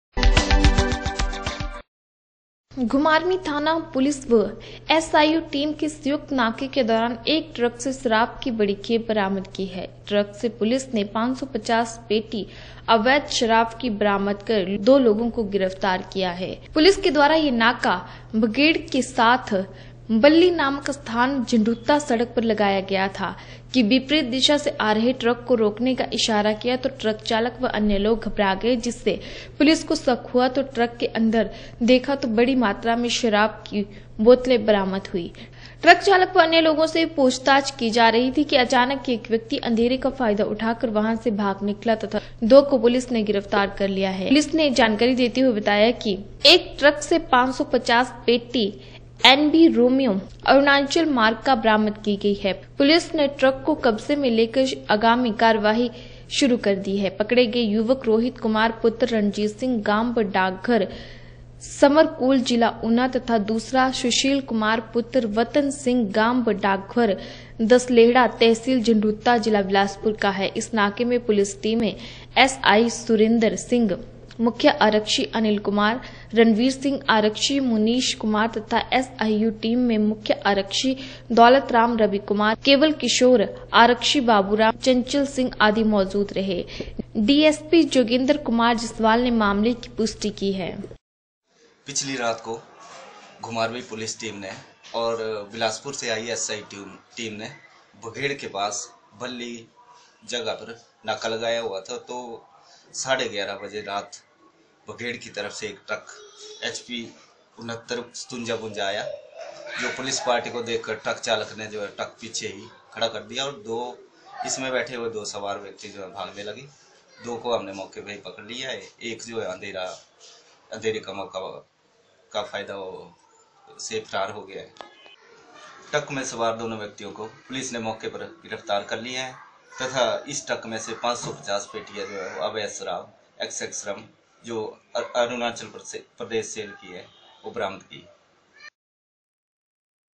घुमारवीं थाना पुलिस व एस आई यू टीम की संयुक्त नाके के दौरान एक ट्रक से आबेद शराब बरामद
घुमारवीं थाना पुलिस व एस आई यू टीम की संयुक्त नाके के दौरान एक ट्रक से शराब की बड़ी खेप बरामद की है  ट्रक से पुलिस ने  550 पेटी आबेद शराब की बरामद दो लोगो को किया गिरफ्दार पुलिस के द्धारा यह नाका भगेड़ के साथ बल्ली नामक स्थान झण्डुता सड़क पर लगाया गया था कि विपरीत दिशा से आ रहे ट्रक को रोकने का इशारा किया तो ट्रक चालक व अन्य लोग घबरा गए जिससे पुलिस को शक हुआ तो ट्रक के अंदर देखा तो बड़ी मात्रा में शराब से लदा थी ।